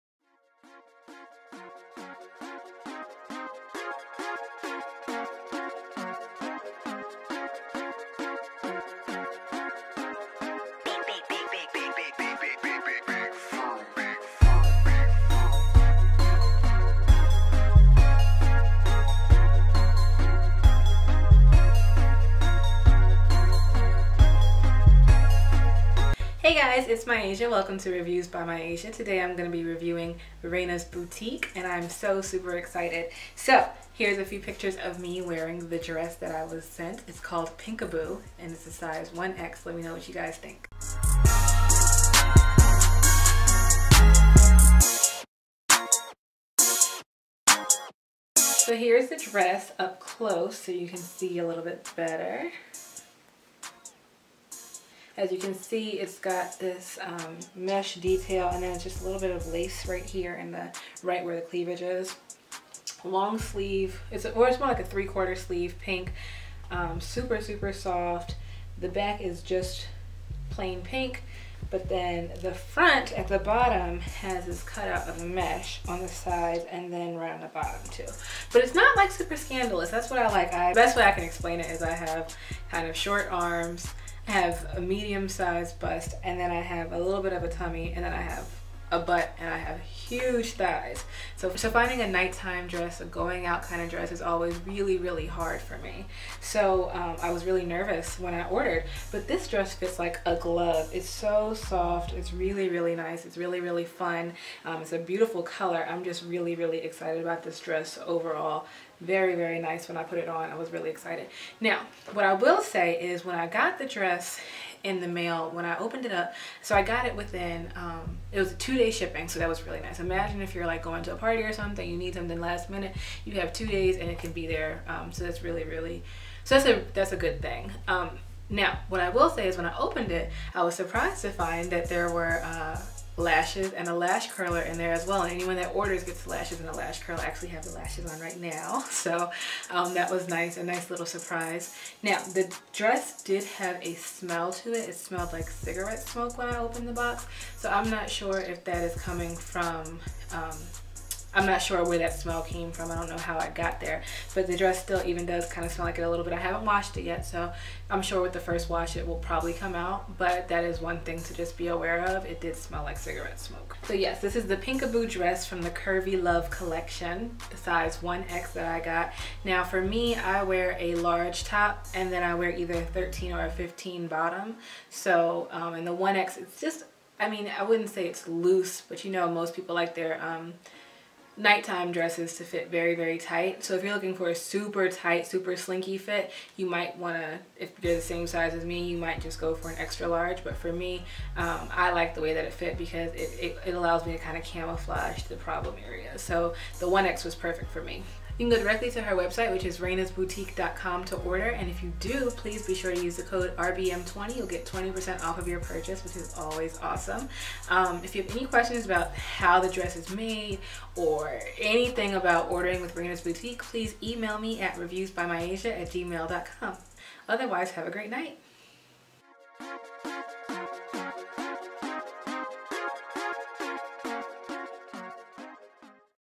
Raenah's Boutique Pink-A-Boo Dress Product Review
Find out what I think about my recent Raenah's Boutique order! Do you like the dress?

--DRESS REVIEWED IN VIDEO--
Raenah's Boutique
Pink-A-Boo

--HAIR WORN IN VIDEO--

PERUVIAN DEEP WAVE
1 - 14" Lace Base Closure
1 - 16" Bundle
1 - 18" Bundle
1 - 20" Bundle

*PRODUCT REVIEW INQUIRIES & GENERAL QUESTIONS:*